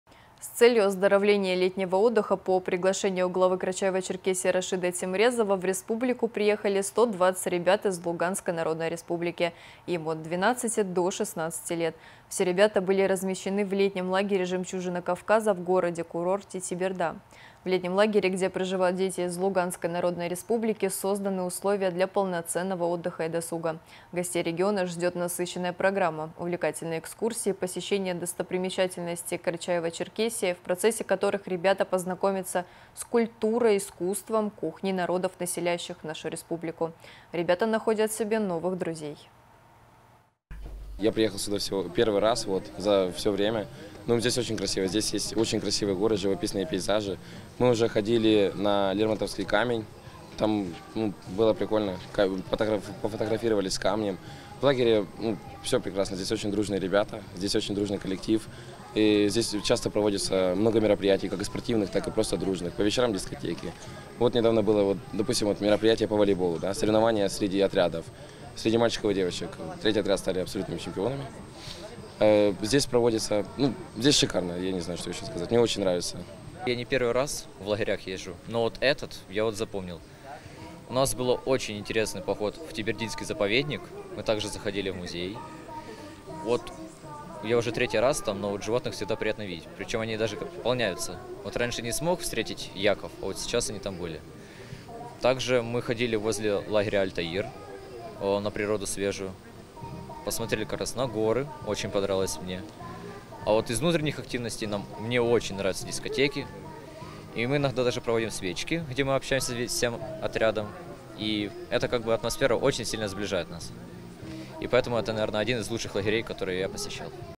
По приглашению главы Республики в Карачаево Черкесию на летний отдых приехали 120 ребят из ЛНР
С целью оздоровления и летнего отдыха, по приглашению главы Карачаево-Черкесии Рашида Темрезова, в Республику приехали 120 ребят из Луганской Народной Республики от 12 до 16 лет.